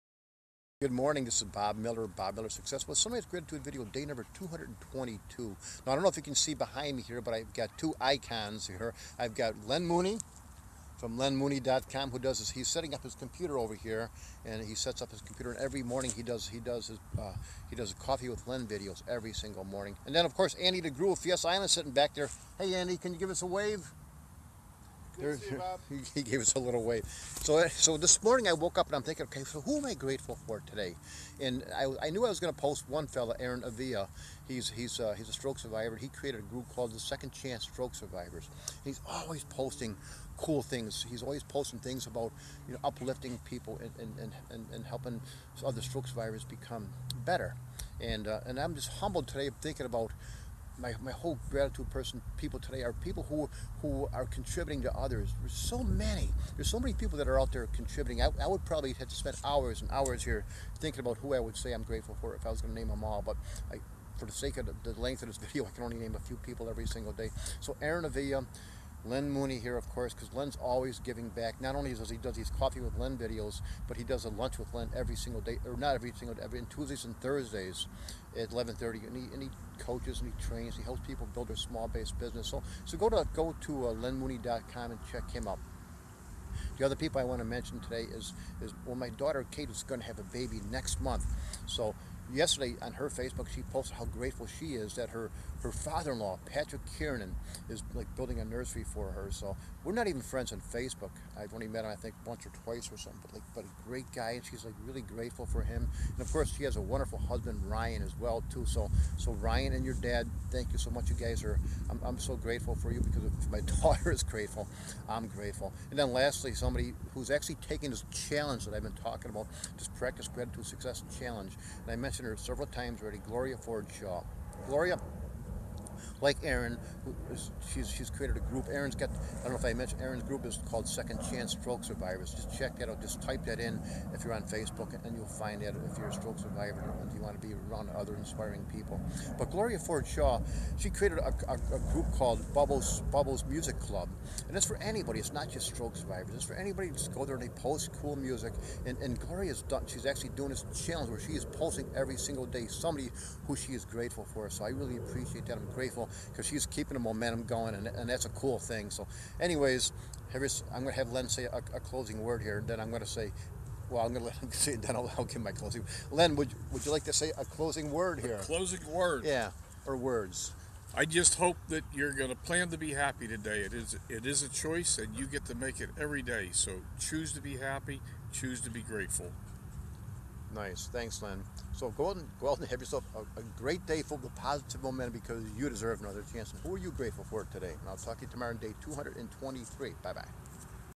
Practice Gratitude Success Sunrise Gratitude Day 222 Len Mooney +
Every day, I create a morning video usually at Sunrise where I name at least one person who I am grateful for each day. Most of my Sunrise Gratitude videos are created on Fiesta Island in San Diego, Ca.
I believe that Practicing Gratitude is one of the keys to Happiness, Abundance and Success.
Today, I am especially Grateful for
Aaron Avila from Second Chance Stroke Survivors,  Ryan Reb Kiernan, Patrick Kiernan, Len Mooney, Gloria Ford Shaw- Bubble's Music Club.